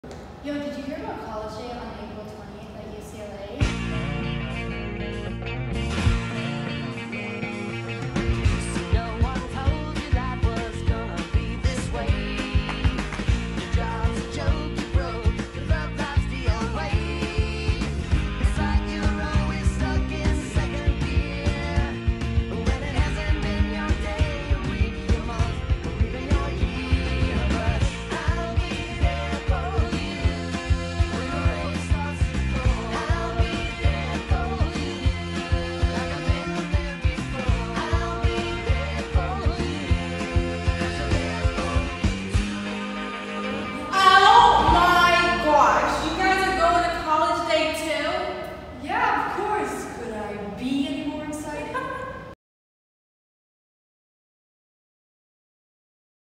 MSA UCLA College Day 2019 Promo Video
Assalamu Alaikum! UCLA MSA's 20th annual college day is coming up soon on April 20th inshaAllah. Don't miss out on this great event!!! Link to register